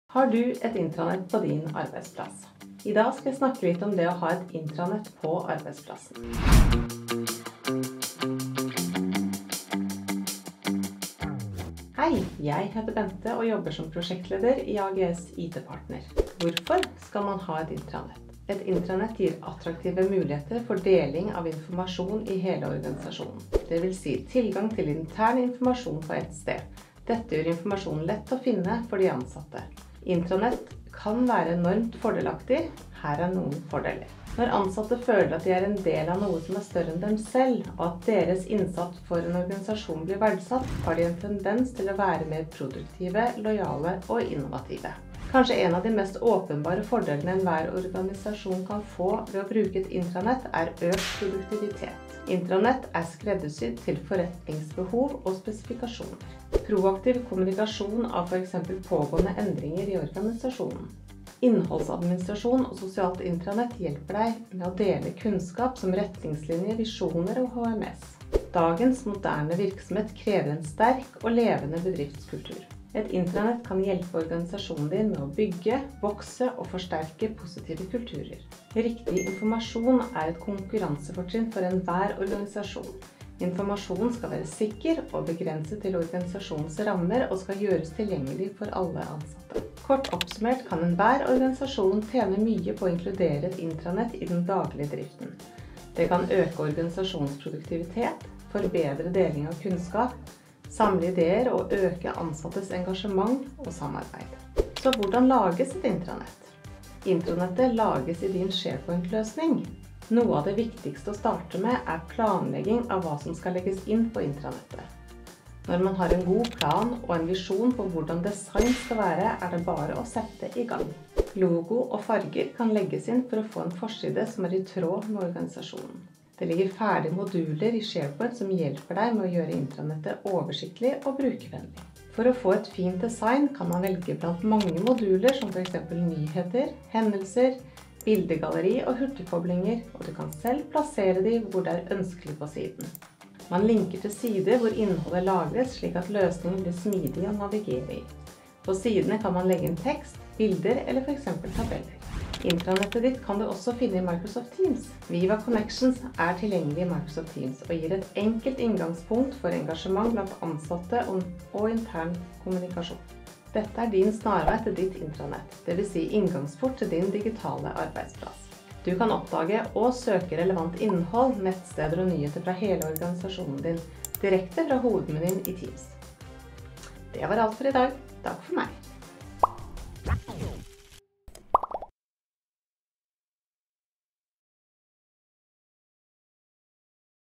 Derfor trenger du et SharePoint - intranett
I videoen vil du få se hvorfor du bør ha et SharePoint intranett på din arbeidsplass!

Et intranett er et internt nettverk for en bedrift, der kun de ansatte i organisasjonen har tilgang.

Et godt intranett sørger for at alle ansatte er oppdatert på det som skjer i bedriften, at de har tilgang til lik informasjon som for eksempel HR informasjon, It policy, elektroniske skjema og organisasjonens visjoner.

00:00 Intro
00:12 Har du intranett på din arbeidsplass?
01:30 kort oppsummert
02:33 Hvor finner du ditt intranett?
02:46 Avslutning